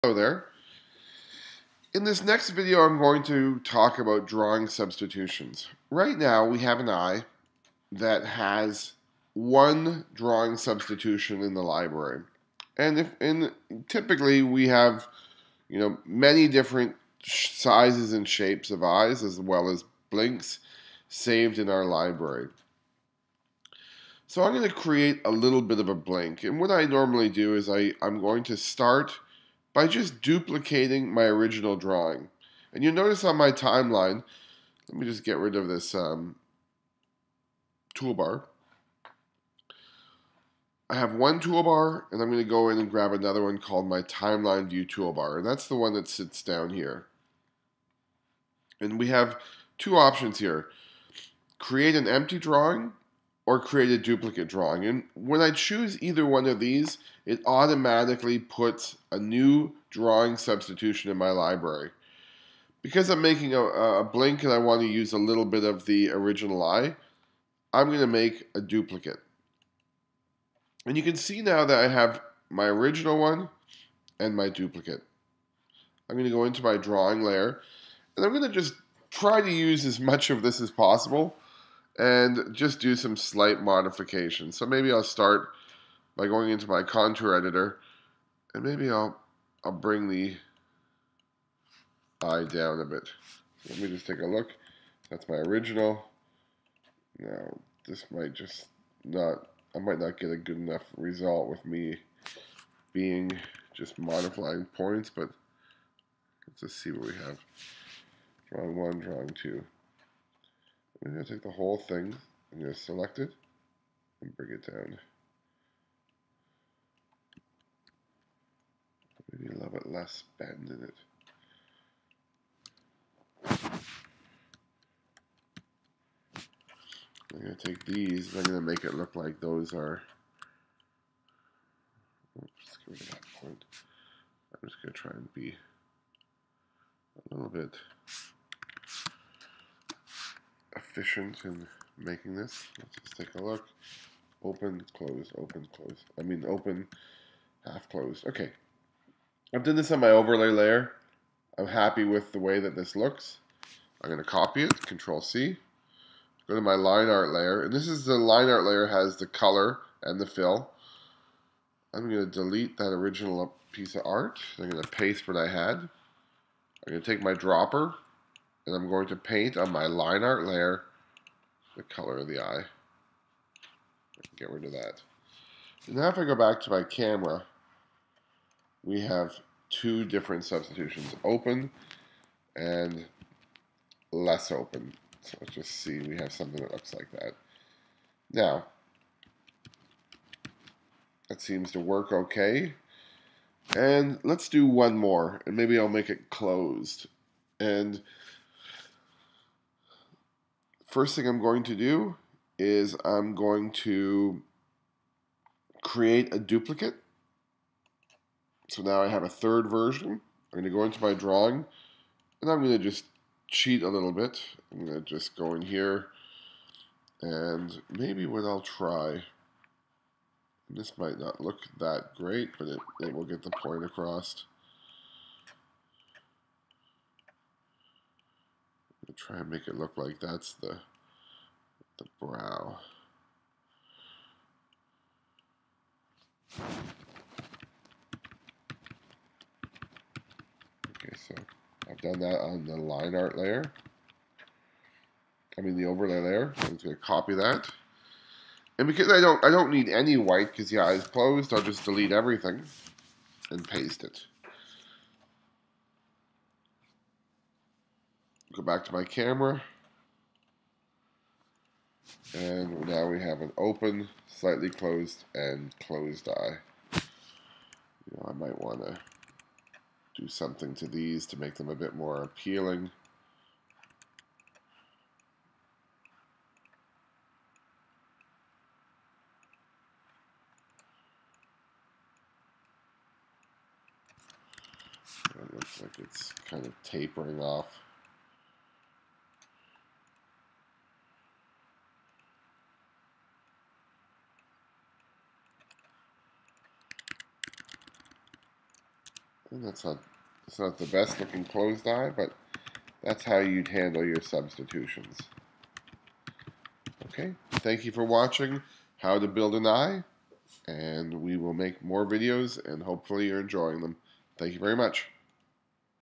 How to Create an Inverted Cut Pupil using Harmony Premium - Part 3
In this topic, you will learn how to build the eyes by invert-cutting the pupils into the eye and creating drawing substitutions to create blinks using Harmony Premium.